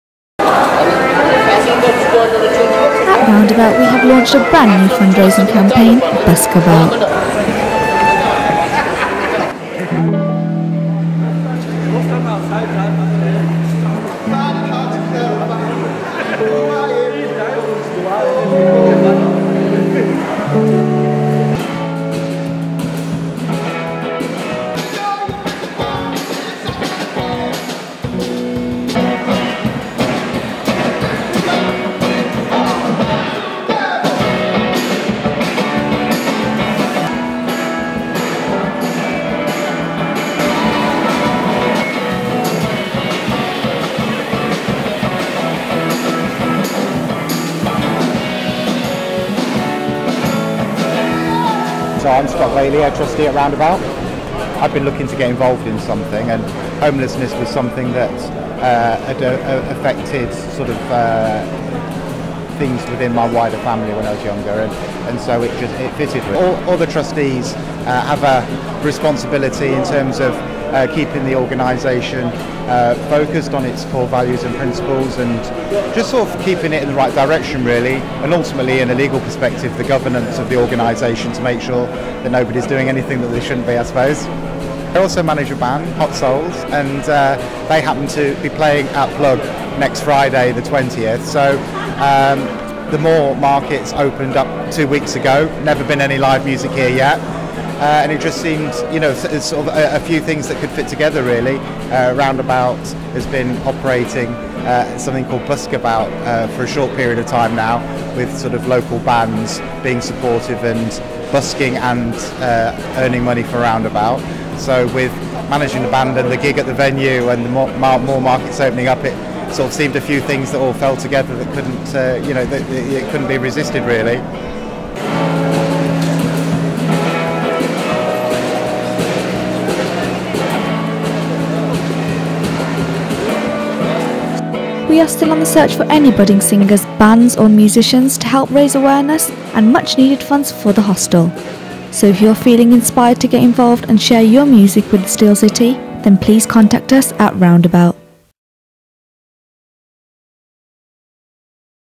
Buskabout with Roundabout
At Roundabout we have launched a new fundraising campaign - BUSKABOUT!

We are asking local singers and bands to sing their hearts out and help raise money to fund the renovation of our hostel.

Fundraising co-ordinator: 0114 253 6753

Thank you for believing in Roundabout and the young people we support.

Produced By Esha Jass
Final year media student at Sheffield Hallam University